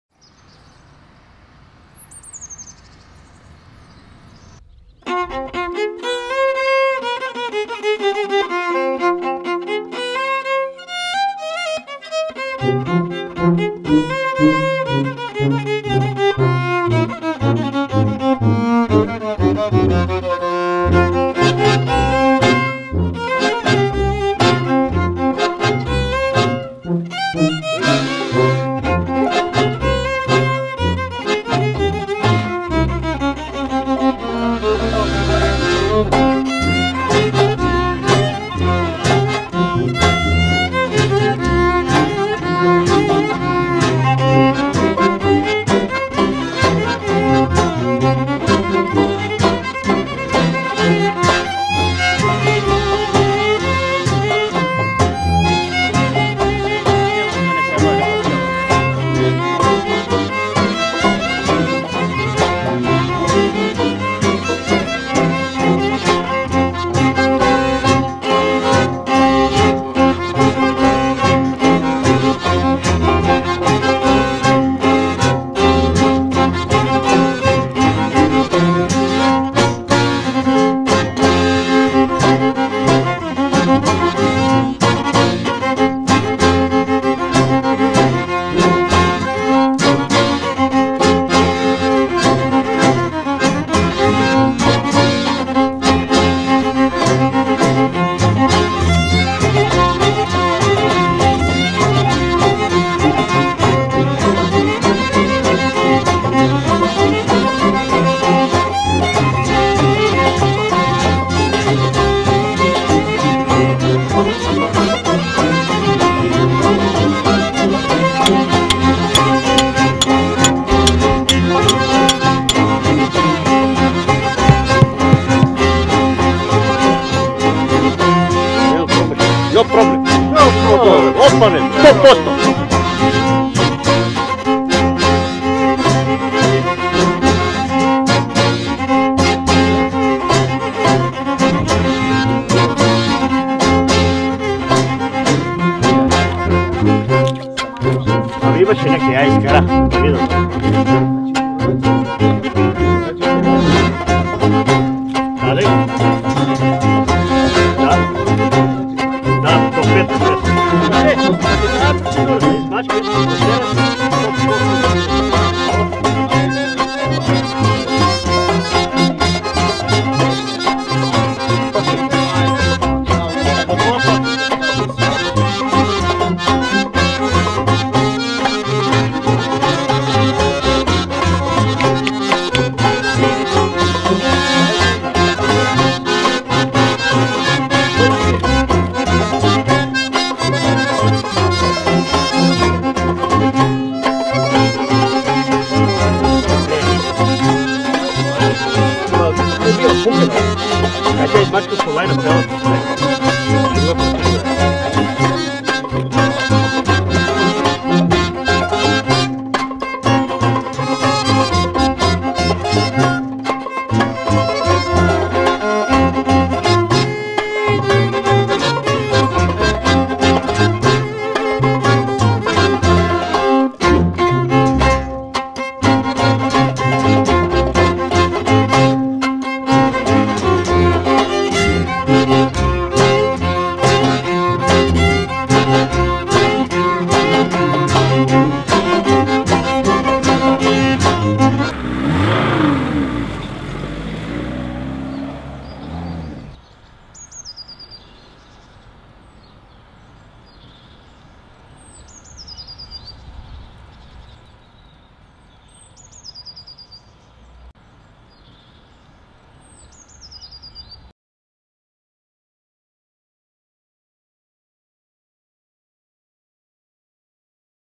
Live and Let Bee North-Macedonia: Koce the rock beekeeper
Koce lives in an abandoned village near Shtip in North-Macedonia. He is a beekeeper. A traditional way to keep his bees, is to let them swarm into the mountains. He makes the holes for them.